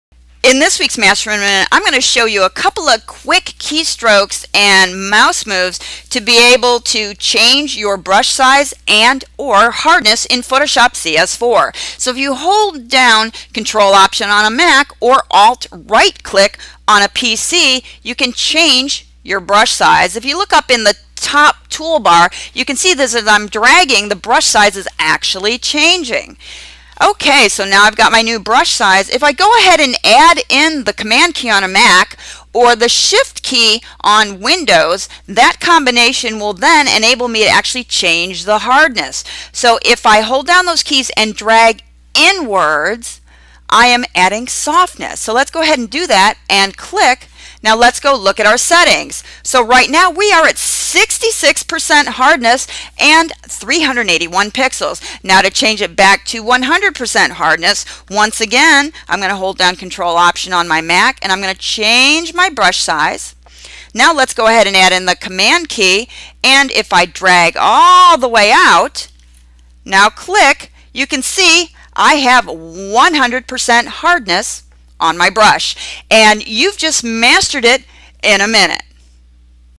Photoshop CS4 Tips
Learn how to quickly change your brush size and hardness in Photoshop CS4.